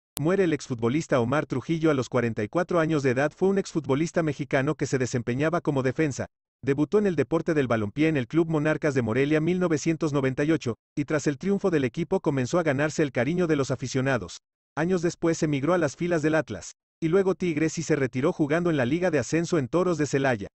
Falleció el ex futbolista Omar Trujillo
Muere el exfutbolista mexicano Omar Trujillo a los 44 años de edad

Causa de muerte: Paro cardíaco

(Falleció el día 1 de diciembre del 2022)